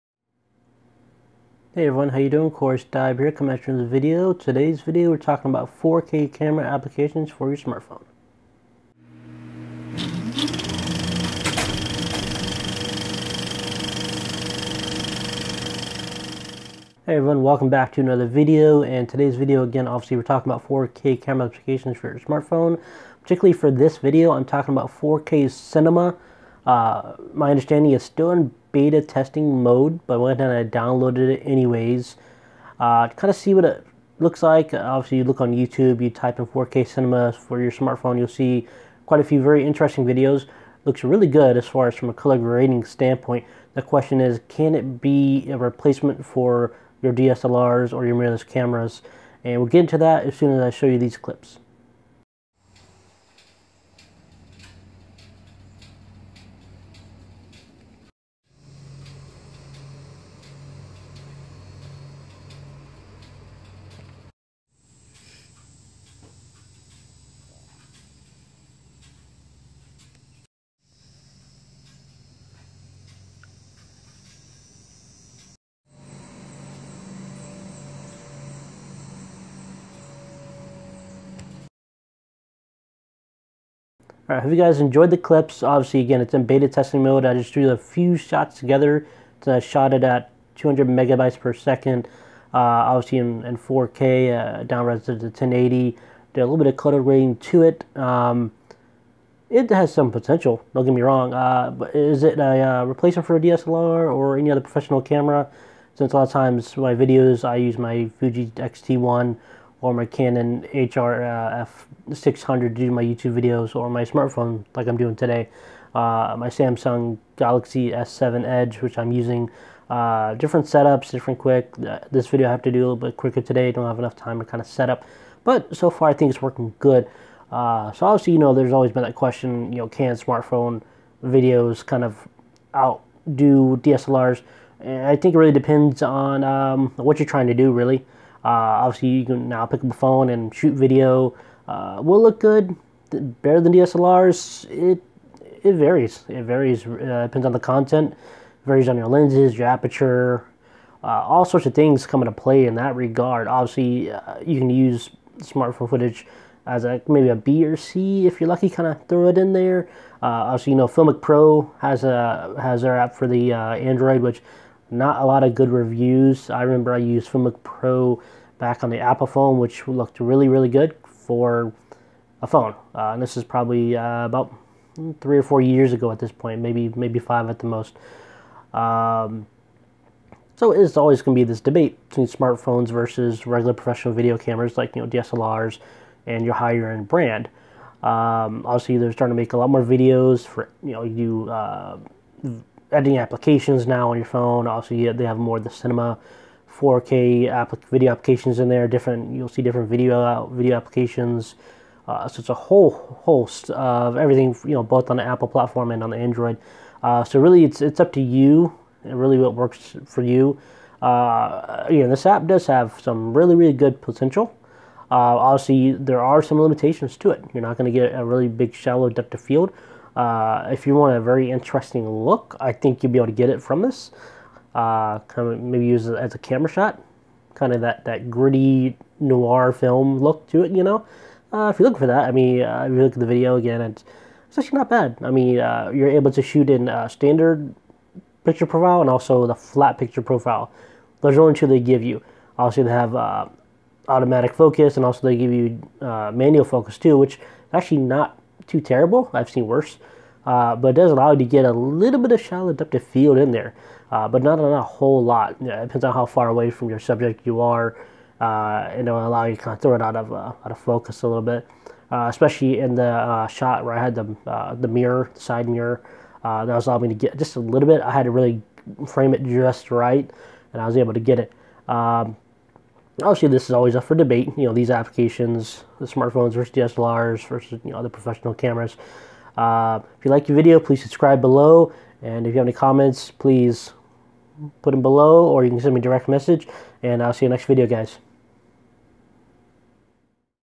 Cinema 4K Video App
Here is a quick video review on the Cinema 4K App in the google play store for Android Phones.